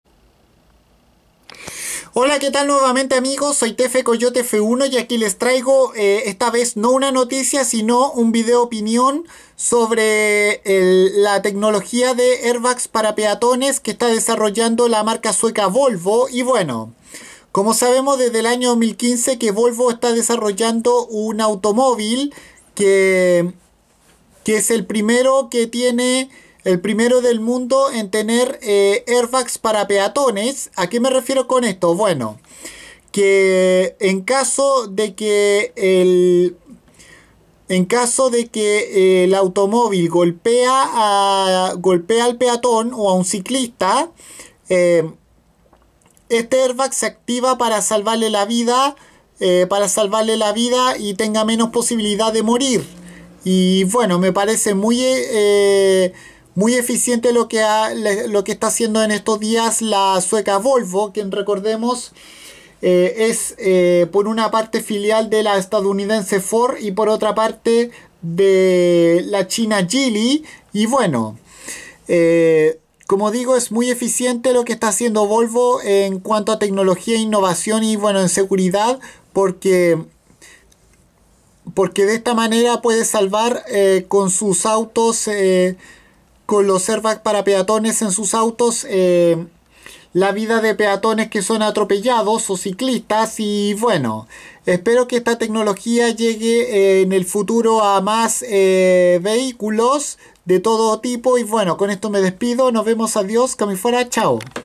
Vídeo opinión sobre los airbags para peatones
En este vídeo les explico mi opinión acerca de los airbags para peatones y ciclistas tecnología en la que innova la sueca Volvo(filial de la china Geely y de la estadounidense Ford Motor Company)